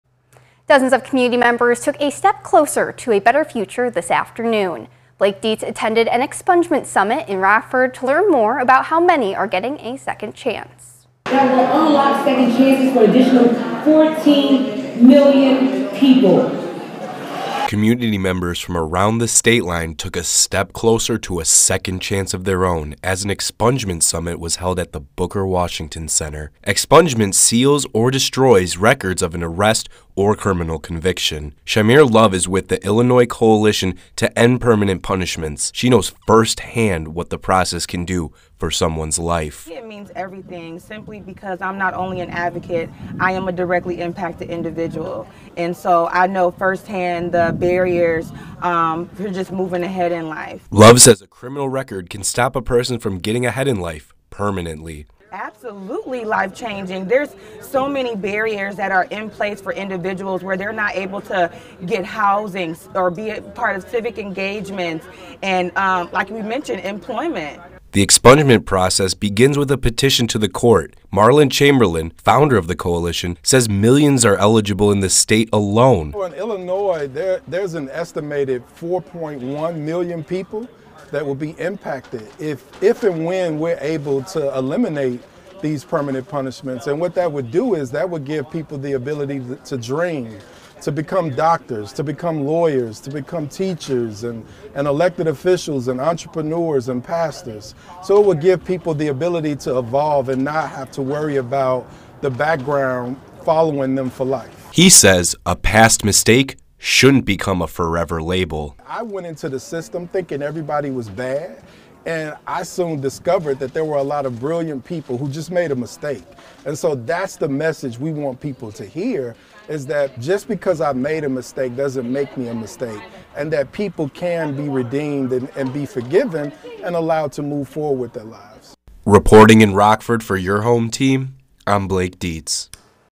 4.1M people in Illinois could be eligible to get their criminal records expunged
The expungement process begins with a petition to the court, Marlon Chamberlain, Founder of the Illinois Coalition to End Permanent Punishment said millions are eligible in the state alone.